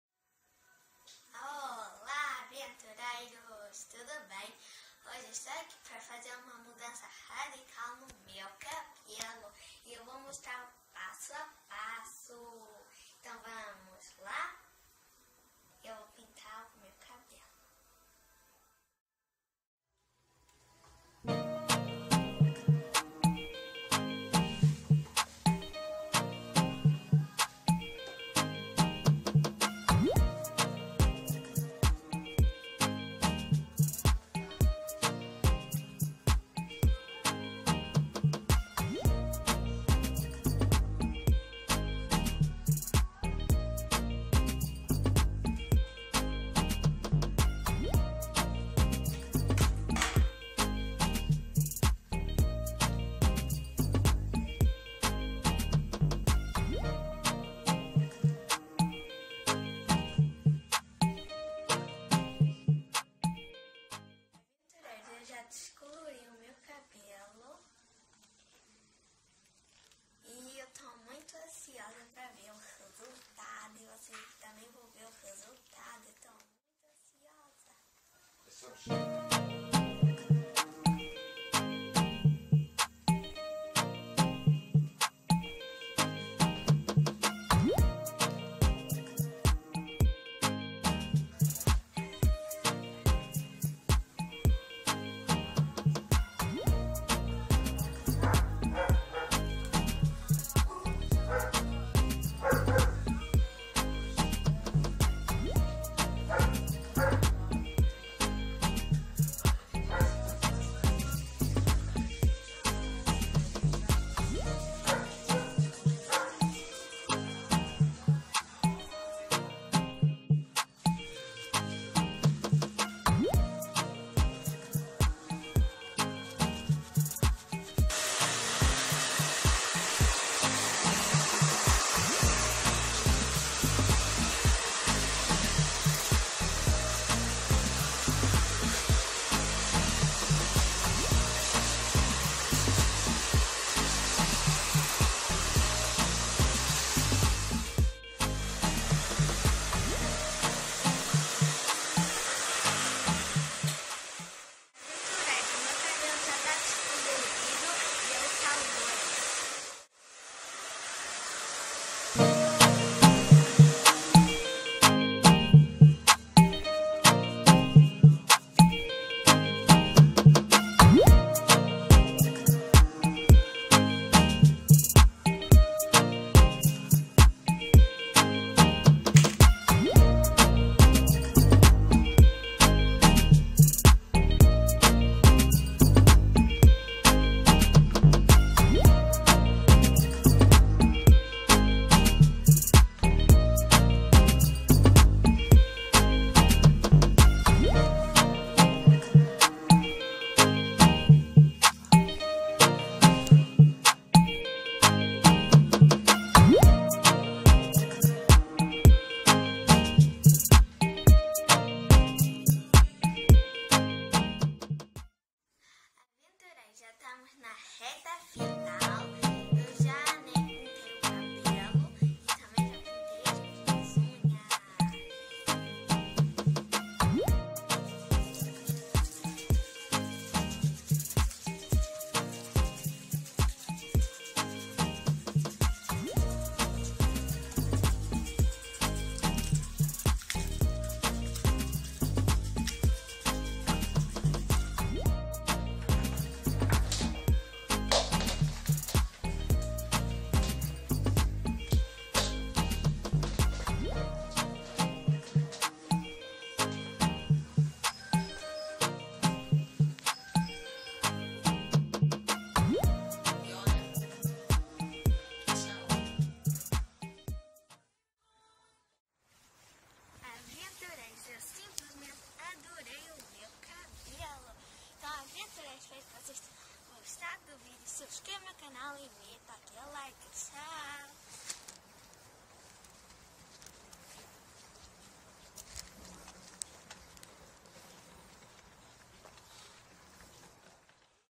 MUDANÇA DE VISUAL /Pintei meu cabelo rosa Pink
Hoje trazemos a mudança de visual da Maria, pintamos o cabelo de rosa Pink.
Se forem fazer alguma mudança faça com um profissional e com autorização dos responsáveis.

Canal para todas as idades 🧓🏼🙎‍♀️👶
100 % Monitorado pelos pais